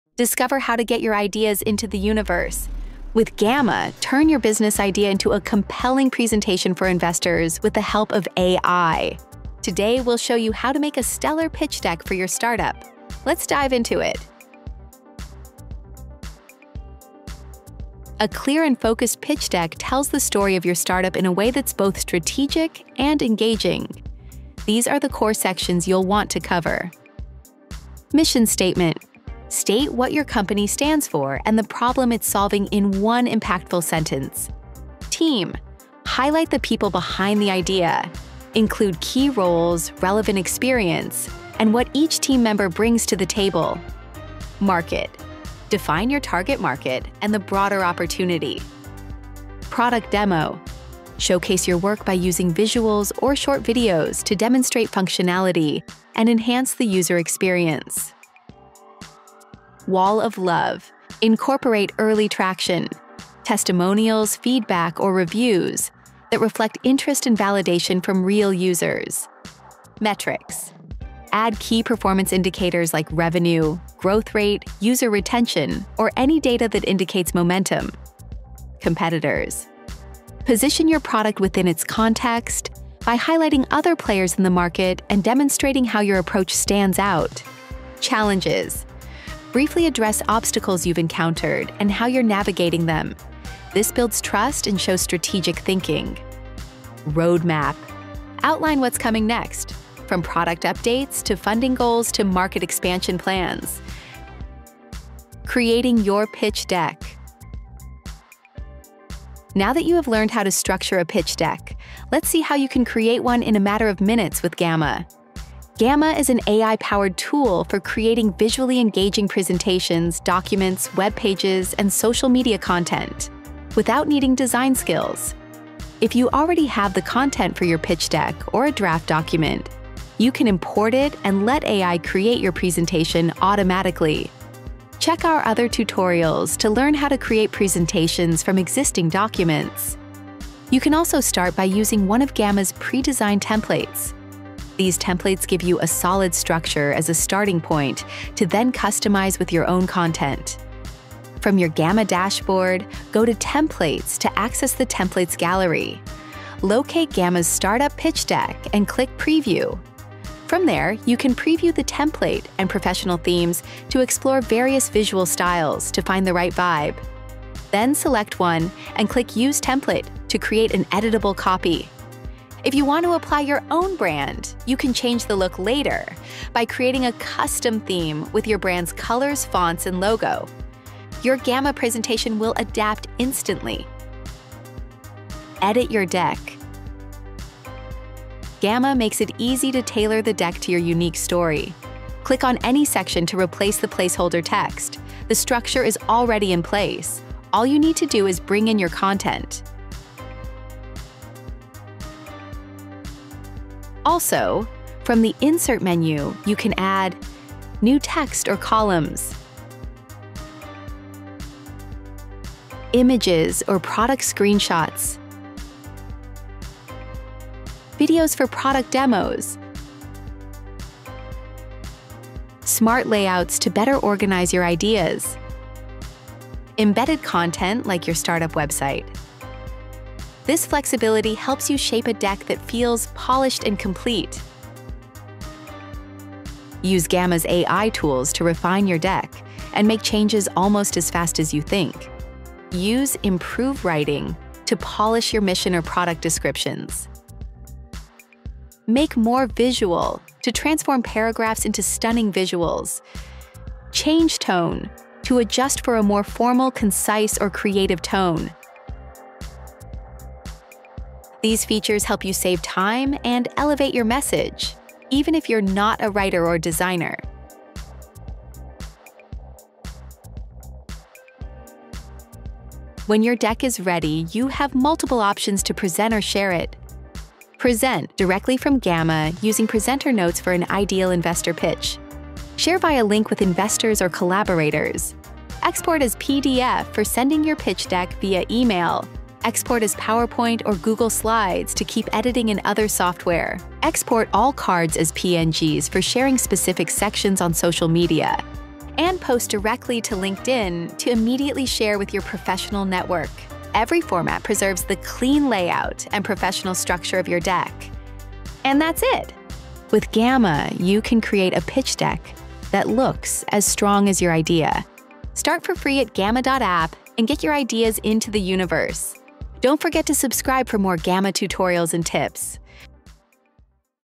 Build a Winning Startup Pitch Deck with AI
Need to pitch your startup idea to investors? In this tutorial, we’ll show you how to build a professional, persuasive pitch deck in minutes using Gamma’s AI-powered tools — no design skills or slide formatting required.
From core pitch structure to using templates, themes, and AI editing, this video walks you through every step to create a deck that tells your story and wins attention.

and build your pitch in minutes.

📌 Chapters:

00:00 - Intro
00:16 - Pitch Deck Structure
01:48 - Creating Your Pitch Deck
03:06 - Edit Your Deck
04:29 - Export and Present
05:08 - Outro

✔️ What You’ll Learn:

✔️ The essential sections of a compelling startup pitch deck
✔️ How to create a deck from templates or existing content
✔️ How to customize layout, branding, and tone
✔️ How to use Gamma’s AI to polish and visualize your ideas
✔️ How to share or export your pitch for any format

✔️ Perfect For:

✔️ Startup founders preparing to pitch
✔️ Accelerators, incubators, and early-stage teams
✔️ Solo entrepreneurs needing fast, pro-level decks
✔️ Anyone turning ideas into investor-ready presentations

and get your ideas into the universe.